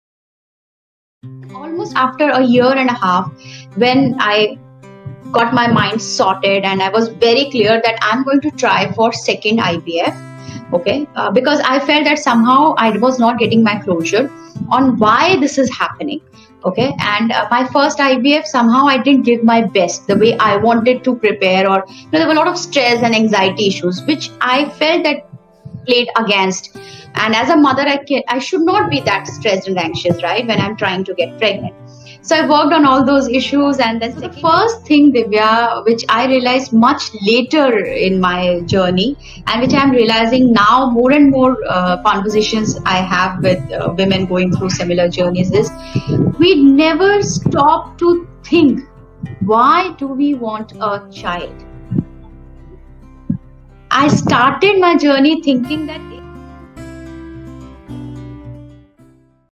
Emotional Aspects of Infertility: Gitanjali Banerjee
Hear Gitanjali talk about the following - ⁠
Emotional Support⁠
The intensity of Medical Procedures⁠
Doubts and Worries about Success⁠